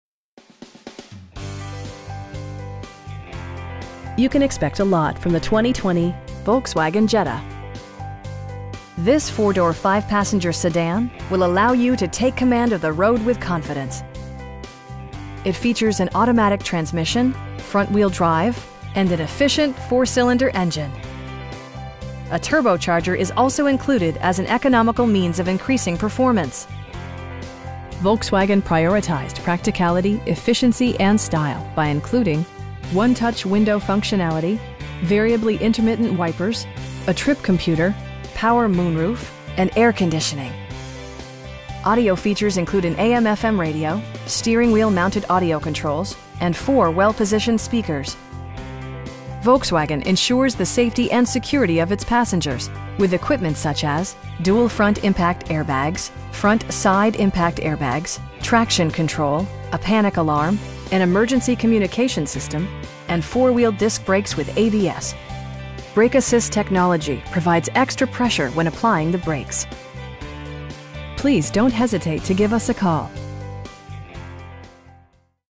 2020 Volkswagen Jetta 1.4T SE w/ULEV in Concord, NC 28027
7500 Hendrick Auto Plaza NW

You can expect a lot from the 2020 Volkswagen Jetta. This 4 door, 5 passenger sedan will allow you to take command of the road with confidence! It features an automatic transmission, front-wheel drive, and an efficient 4 cylinder engine. A turbocharger is also included as an economical means of increasing performance. Volkswagen prioritized practicality, efficiency, and style by including: 1-touch window functionality, variably intermittent wipers, a trip computer, power moon roof, power door mirrors, an overhead console, and air conditioning. Audio features include an AM/FM radio, steering wheel mounted audio controls, and 4 speakers, providing excellent sound throughout the cabin. Volkswagen ensures the safety and security of its passengers with equipment such as: dual front impact airbags, front side impact airbags, traction control, a panic alarm, an emergency communication system, and 4 wheel disc brakes with ABS. Brake assist technology provides extra pressure when applying the brakes. We know that you have high expectations, and we enjoy the challenge of meeting and exceeding them! Please don't hesitate to give us a call.